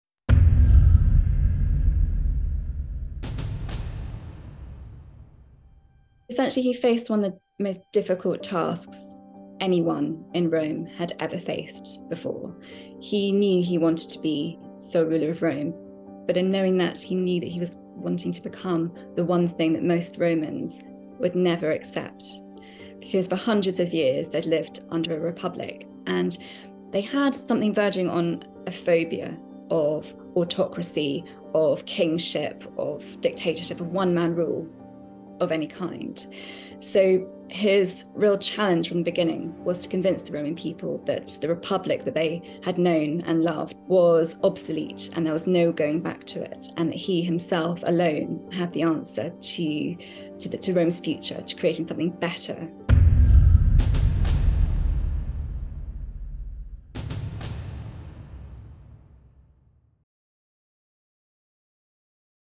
Becoming Emperor
Historian and author Dr Daisy Dunn summarises how Octavian navigated the politics of Ancient Rome to become the first Roman Emperor - an all powerful dictator posing as a man of the people.

Editor Matt Platts-Mills experiments with AI art generator Midjourney to create cinematic historical stills of Augustus.